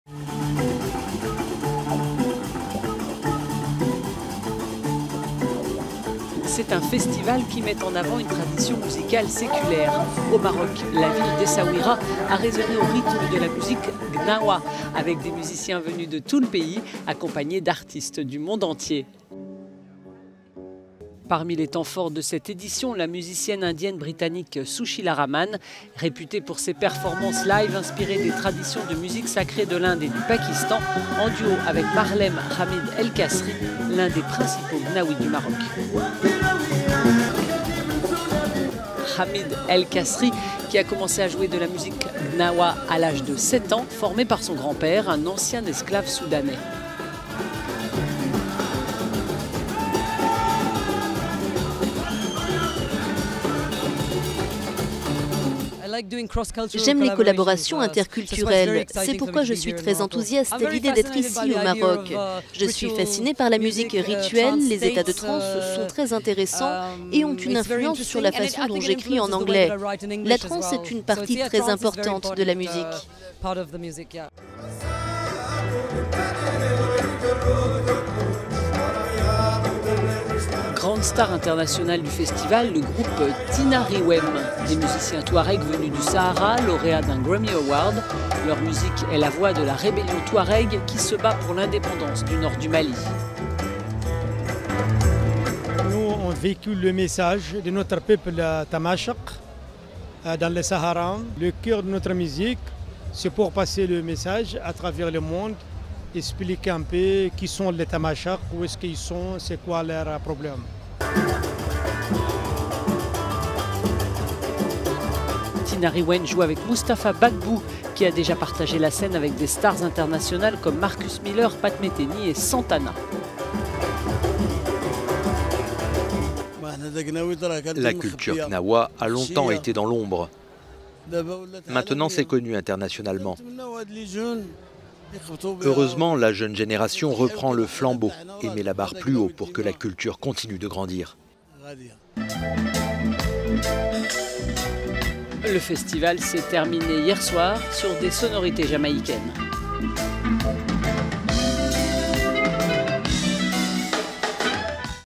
La musique gnaoua met le Maroc en transe
La musique gnaoua met le Maroc en transe. Plusieurs stars internationales se sont partagées la scène du festival d'Essaouira pour la 22e édition d'un évènement consacré à une tradition musicale séculaire.  …